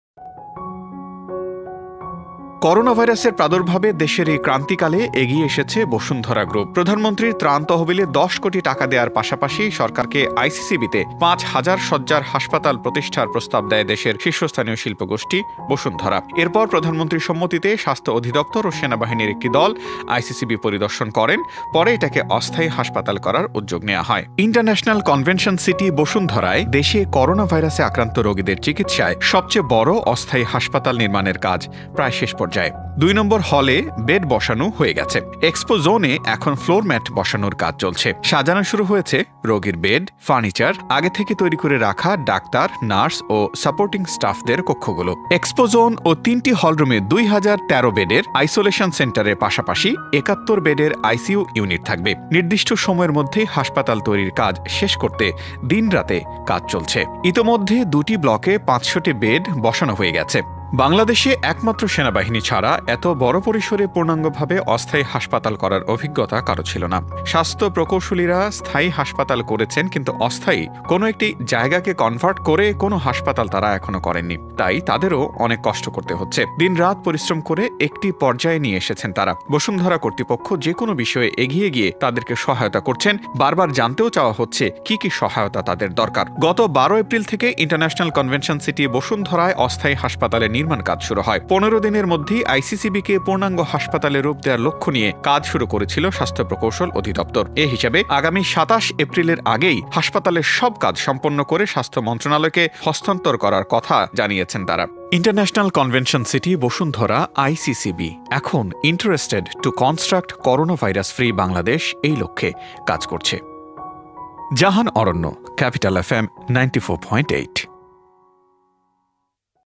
দেশ ও মানুষের কল্যাণে করোনা চিকিৎসায় নির্দিষ্ট সময়েই কাজ শুরু করবে বসুন্ধরার হাসপাতাল
ইন্টারন্যাশনাল কনভেনশন সিটি বসুন্ধরায় (আইসিসিবি) দেশে করোনাভাইরাসে আক্রান্ত রোগীদের চিকিৎসায় দেশের সবচেয়ে বড় অস্থায়ী হাসপাতাল নির্মাণের কাজ প্রায় শেষ পর্যায়ে। দুই নম্বর হলে বেড বসানো হয়ে গেছে। এক্সপো জোনে এখন ফ্লোরম্যাট বসানোর কাজ চলছে। সাজানো শুরু হয়েছে রোগীর বেড, ফার্ণিচার, আগে থেকে তৈরি করে রাখা ডাক্তার, নার্স ও সার্পোটিং স্টাফদের কক্ষগুলো। এক্সপো জোন ও তিনটি হলরুমে দুই হাজার ১৩ বেডের আইসোলেশন সেন্টারের পাশাপাশি ৭১ বেডের আইসিইউ ইউনিট থাকবে। নির্দিষ্ট সময়ের মধ্যেই হাসপাতাল তৈরির কাজ শেষ করতে দিনে-রাতে চলছে কাজ।

আজ বৃহস্পতিবার (২৩ এপ্রিল) আইসিসিবি’তে করোনা রোগীদের জন্য নির্মাণাধীন হাসপাতালের কাজের অগ্রগতির কথা তুলে ধরেন আইসিসিবির প্রধান পরিচালন কর্মকর্তা এম এম জসীম উদ্দিন। তিনি জানান, ইতোমধ্যে দুটি ব্লকে ৫০০টি বেড বসানো হয়ে গেছে। আগামীকালের ভেতরে আরো ৫০০টি বেড বসানো হবে। স্বাস্থ্য প্রকৌশলী অধিদপ্তর যারা আছে তাদের সাথে আমরা কথা বলে জানতে পারলাম। গত ১২ এপ্রিল ঘোষণা করা হয়েছিল, আগামী ১৫ দিনের মধ্যেই আমরা হাসপাতালের কার্যক্রমে যেতে চাই। সেই লক্ষ্য নিয়ে কাজ শুরু করে স্বাস্থ্য প্রকৌশল অধিদপ্তর, সরকার ও বসুন্ধরা গ্রুপ একই সাথে সমন্বয় করে কাজগুলো করে যাচ্ছি। আমরা সন্তুষ্ট যে এ পর্যন্ত আসতে পেরেছি। দেড়শ পাকা টয়লেট তৈরি করা কিন্তু অনেক কঠিন কাজ, আমাদের এ ট্রেন্ডটা ছিল বলেই বসুন্ধরা গ্রুপ এটা দিতে পারছে। আগামী ২৭-২৮ তারিখের মধ্যে রোগী সেবার জন্য এটা প্রস্তুত থাকবে বলে আশা করি। আমরা মন্ত্রণালয় থেকে যেটা জেনেছি তাদের সিদ্ধান্ত হলো যে হাসপাতলের শতভাগ কাজ সম্পূর্ণ কাজ হলে সেবার জন্য খোলা যাবে। এটা যেহেতু ভাইরাস স্ট্রিটমেন্ট হবে। চিকিৎসা চলাকালীন কোনো নির্মাণ কাজ করা যাবে না। সেই সূত্র ধরে আমরা বলতে পারি আমাদের যে প্রস্তুতি এটা একেবারে দ্বারপ্রান্তে, সকল সরঞ্জাম এখানে চলে আচ্ছে এখন শুধু এগুলোকে স্থাপন করা হবে।

তিনি বলেন, সরকারের যে মূল পরিকল্পনা ছিল সেবা দেওয়া সে পরিকল্পনাটি এখন বাস্তবে মুখ দেখছে। বাংলাদেশের প্রেক্ষাপটে এ ধরনের ইনফাস্ট্রাকচার না থাকলে হয়তো বা সরকার এ সিদ্ধান্ত টা জরুরিভাবে পেত না। এ জন্য আমাদের বসুন্ধরার চেয়ারম্যান সাহেবকে আবারো ধন্যবাদ জানাই এ জন্য যে তিনি দ্রুত বুঝিয়ে দিতে পেরেছেন। আমাদের বাণিজ্যিক কার্যক্রম নেই, আমরা এক দেড়'শ লোক সবসময় উনারা যেভাবে বলছেন সেভাবে কাজ করে যাচ্ছি।

গতকাল তিনি জানিয়েছিলেন, বাংলাদেশে একমাত্র সেনাবাহিনী ছাড়া এর বড় পরিসরে পূর্ণাঙ্গভাবে অস্থায়ী হাসপাতাল করার অভিজ্ঞতা কারো ছিল না। স্বাস্থ্য প্রকৌশলীরা স্থায়ী হাসপাতাল করেছেন, কিন্তু অস্থায়ী কোনো একটি জায়গাকে কনভার্ট করে কোনো হাসপাতাল তারা এখনো করেনি। তাই তাদেরও অনেক কষ্ট করতে হচ্ছে। দিন-রাত পরিশ্রম করে একটি পর্যায়ে নিয়ে এসেছেন তারা। বসুন্ধরা কর্তৃপক্ষ যেকোনো বিষয়ে এগিয়ে গিয়ে তাদেরকে সহায়তা করছে। বারবার জানতেও চাওয়া হচ্ছে কি কি সহায়তা তাদের দরকার।

গত ১২ এপ্রিল থেকে ইন্টারন্যাশনাল কনভেনশন সিটি বসুন্ধরায় (আইসিসিবি) অস্থায়ী হাসপাতালের নির্মাণ কাজ শুরু হয়। ১৫ দিনের মধ্যেই আইসিসিবিকে পূর্ণাঙ্গ হাসপাতালে রূপ দেওয়ার লক্ষ্য নিয়ে কাজ শুরু করেছিলেন স্বাস্থ্য প্রকৌশল অধিদপ্তর। এ হিসেবে আগামী ২৭ এপ্রিলের আগেই হাসপাতালের সবকাজ সম্পন্ন করে স্বাস্থ্য মন্ত্রণালয়কে হস্তান্তর করার কথা। দিন-রাত কাজ হচ্ছে।

উল্লেখ্য, করোনা ভাইরাসের প্রার্দুভাবে দেশের এই ক্রান্তিকালে এগিয়ে এসেছে বসুন্ধরা গ্রুপ। প্রধানমন্ত্রীর ত্রাণ তহবিলে দশ কোটি টাকা দেওয়ার পাশাপাশি সরকারকে আইসিসিবিতে পাঁচ হাজার শয্যার হাসপাতাল প্রতিষ্ঠার প্রস্তাব দেয় দেশের শীর্ষস্থানীয় শিল্প গোষ্ঠী বসুন্ধরা। এর পর প্রধানমন্ত্রীর সম্মতিতে স্বাস্থ্য অধিদপ্তর ও সেনাবাহিনীর একটি দল আইসিসিবি পরিদর্শন করে। পরে এটাকে অস্থায়ী হাসপাতাল করার উদ্যোগ নেওয়া হয়। নানা হিসাব-নিকাশ, পরীক্ষা-নিরীক্ষা শেষে সেখানে দুই হাজার ১৩ শয্যার আইসোলেশন সেন্টার স্থাপনের কাজ করছে। এর বাইরে সেখানে হবে ৭১ বেডের আইসিইউ ইউনিট। আইসিসিবির সুবিশাল চারটি কনভেনশন হল ও একটি এক্সপো জোন, দেশের অন্যতম বৃহত্ এ হাসপাতালটির নির্মাণকাজ বাস্তবায়ন করছে স্বাস্থ্য প্রকৌশল অধিদপ্তর। যত দিন এ মহামারি থাকবে এবং সরকারের যত দিন ব্যবহারের প্রয়োজন শেষ না হবে তত দিন আইসিসিবিকে ব্যবহারের জন্য দেওয়া হয়েছে।